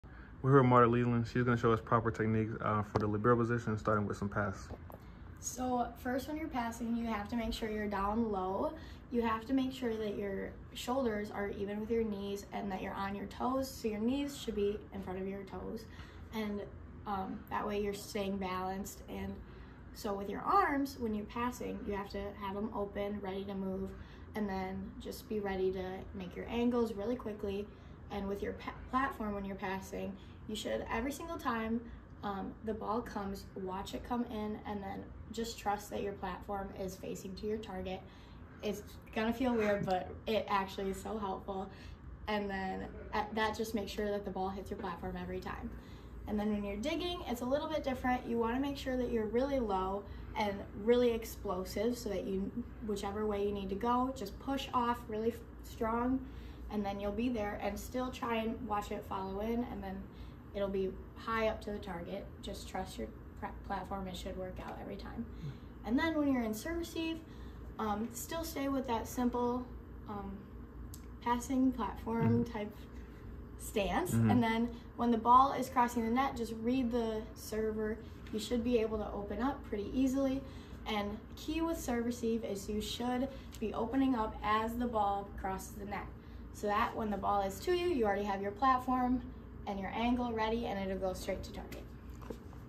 How does a libero pass the ball? Portage's Marta Leeland breaks it down
Portage junior Marta Leeland explains the proper technique to use when playing the libero position.